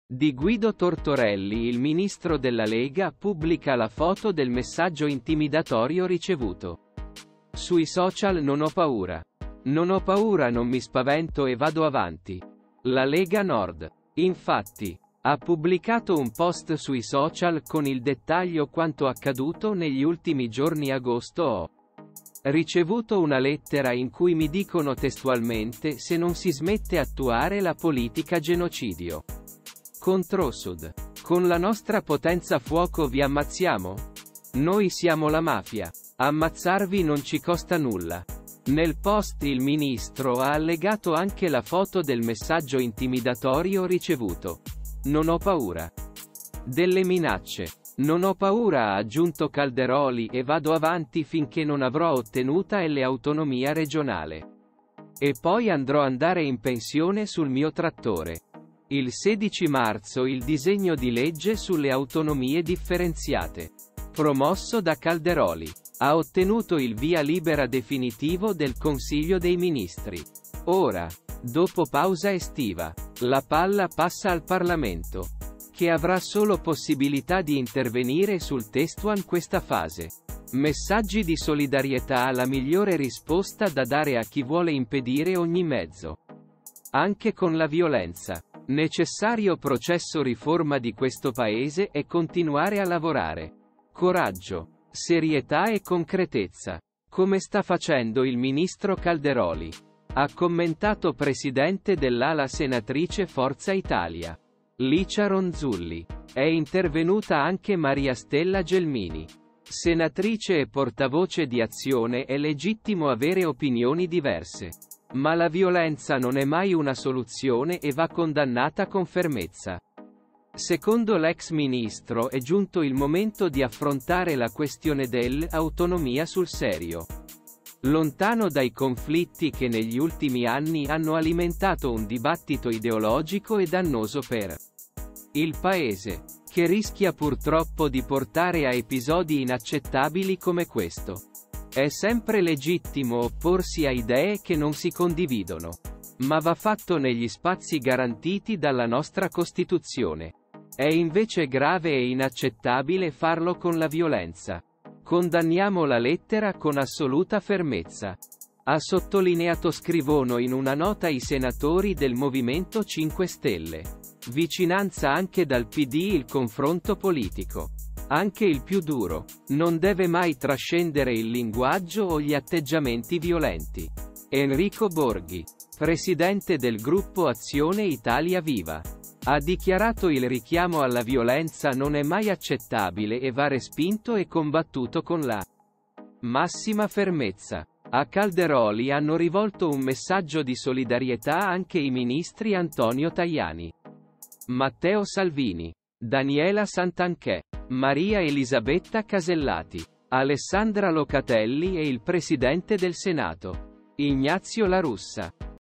Autonomia, Calderoli "Ho minacciato di morte, ma vado avanti finché non sarà fatta"
di Guido Tortorelli  Il ministro della Lega pubblica la foto del messaggio intimidatorio ricevuto sui social "Non ho paura, non ho paura" "Non mi spavento e vado avanti". La Lega Nord, infatti, ha pubblicato un post sui social con il dettaglio quanto accaduto "Negli ultimi giorni agosto ho ricevuto una lettera in cui mi dicono testualmente" se non si smette attuare la politica genocidio controSud, con la nostra potenza fuoco vi ammazziamo? Noi siamo la mafia, ammazzarvi non ci costa nulla". Nel post il ministro ha allegato anche la foto del messaggio intimidatorio ricevuto.«Non ho paura delle minacce, non ho paura - ha aggiunto Calderoli - e vado avanti finché non avrò ottenuta l'autonomia regionale. E poi andròandare in pensione sul mio trattore». Il 16 marzo il disegno di legge sulle autonomie differenziate, promosso da Calderoli, ha ottenuto il via libera definitivo del Consiglio dei ministri. Ora, dopo pausa estiva, la palla passa al Parlamento, che avrà solo possibilità di intervenire sul testoin questa fase. Messaggi di solidarietà "La migliore risposta da dare a chi vuole impedire ogni mezzo, anche con la violenza, necessario processo riforma di questo Paese è continuare a lavorare, coraggio, serietà e concretezza, come sta facendo il Ministro Calderoli", ha commentato presidente dellala senatrice Forza Italia, Licia Ronzulli. È intervenuta anche Mariastella Gelmini, senatrice e portavoce di Azione "È legittimo avere opinioni diverse, ma la violenza non è mai una soluzione e va condannata con fermezza". Secondo l'ex ministro è giunto il momento di "affrontare la questione"dell’autonomia sul serio, lontano dai conflitti che negli ultimi anni hanno alimentato un dibattito ideologico e dannoso per il Paese, che rischia purtroppo di portare a episodi inaccettabili come questo».». «È sempre legittimo opporsi a idee che non si condividono, ma va fatto negli spazi garantiti dalla nostra Costituzione. È invece grave e inaccettabile farlo con la violenza. Condanniamo la lettera con assoluta fermezza», ha sottolineatoscrivono in una nota i senatori del Movimento 5 Stelle. Vicinanza anche dal Pd "Il confronto politico, anche il più duro, non deve mai trascendere il linguaggio o gli atteggiamenti violenti" . Enrico Borghi, presidente del gruppo Azione-Italia viva, ha dichiarato "Il richiamo alla violenza non è mai accettabile e va respinto e combattuto con la massima fermezza". A Calderoli hanno rivolto un messaggio di solidarietà anche i ministri Antonio Tajani, Matteo Salvini, Daniela Santanchè, Maria Elisabetta Casellati, Alessandra Locatelli e il presidente del Senato, Ignazio La Russa.